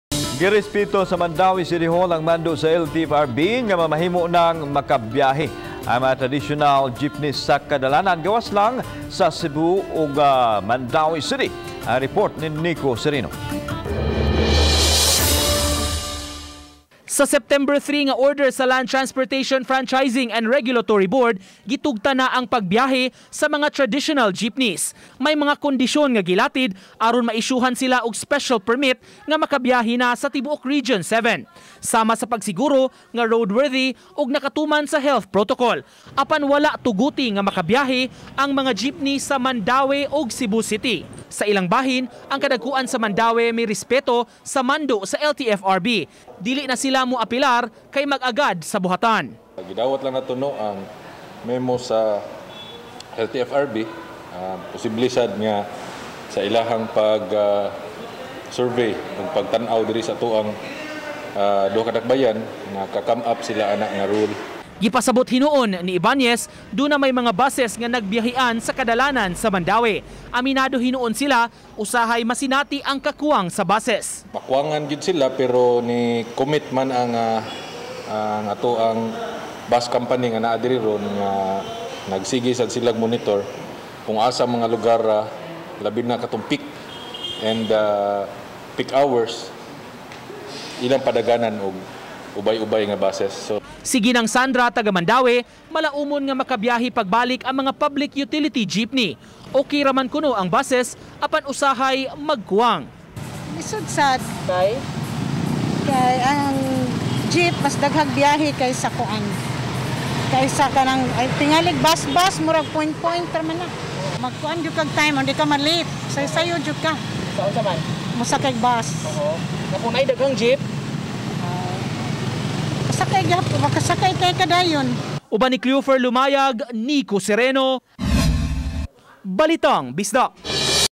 Balitang Bisdak: Walay Gitugtan nga Traditional PUJ nga Makabiyahe sa Mandaue City
Kon sa ubang lugar gitugot na, apan sa Mandaue City ug Cebu City wala pa itugot sa LTFRB-7  nga makabiyahe ang traditional PUJs.